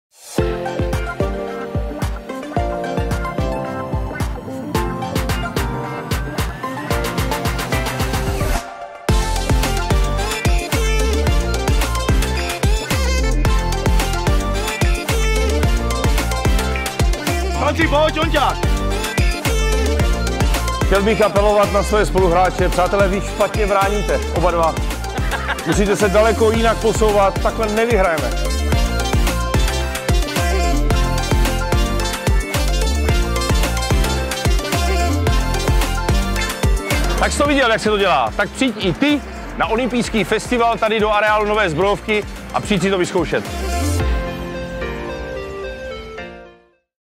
Exhibiční zápas ženské reprezentace U16
Ženský hokejový tým U16 vyzval Petra Švancaru, Luďka Zelenku a Jaroslava Soukupa. Jak to dopadlo?